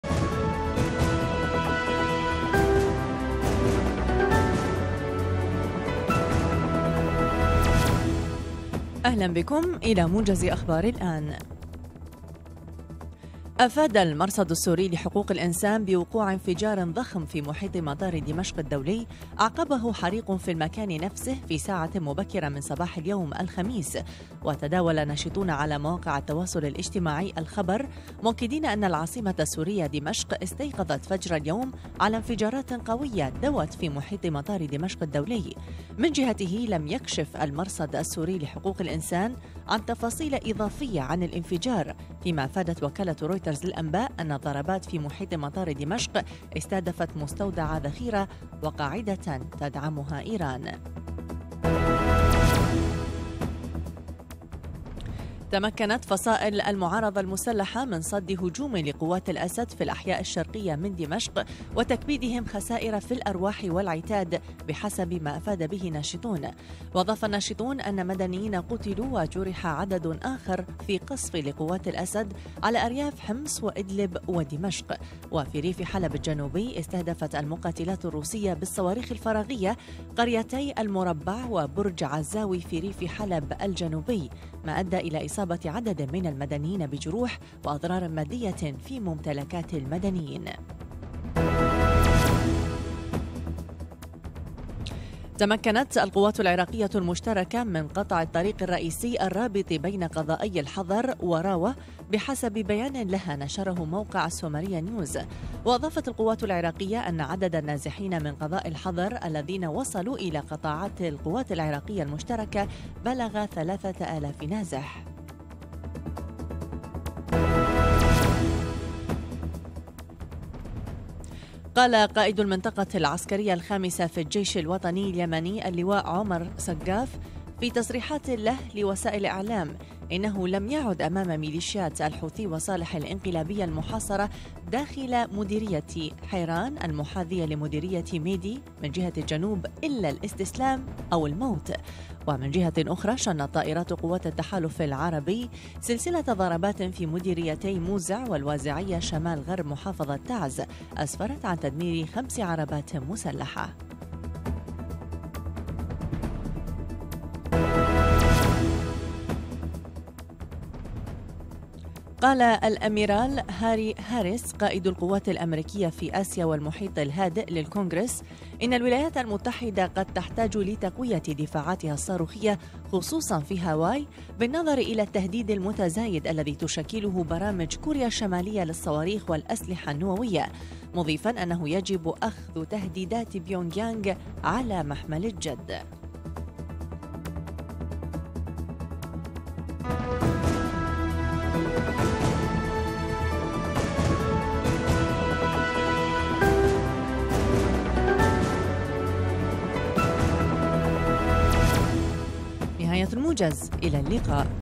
27-4-2017 | موجز التاسعة لأهم الأخبار من
أبرز العناوين:
- سماع دوي انفجارات في محيط مطار دمشق الدولي
- المعارضة تصد هجوما لقوات الاسد شرقي دمشق
- القوات العراقية المشتركة تقطع الطريق الرابط بين الحضر وراوة
- قوات الشرعية اليمنية تحاصر الحوثيين في
- قائد أمريكي: واشنطن بحاجة لدفاع أقوى ضد صواريخ بيونغ يانغ